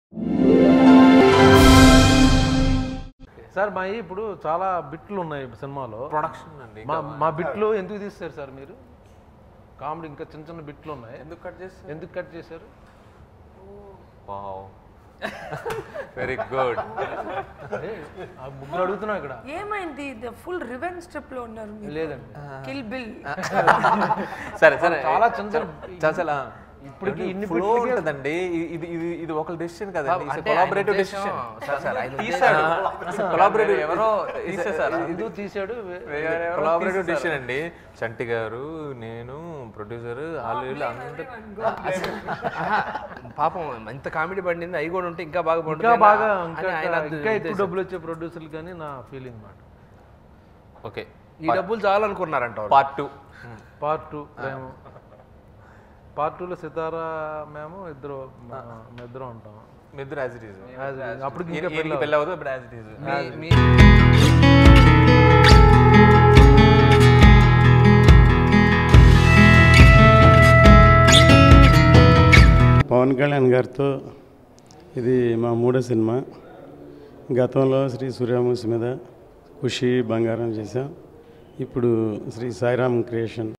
Premam Team Funny chit chat | Naga Chaitanya | Shruthi Hassan | Chandoo | E3 Talkies
Watch  Premam Team Funny chit chat | Naga Chaitanya | Shruthi Hassan | Chandoo | E3 Talkies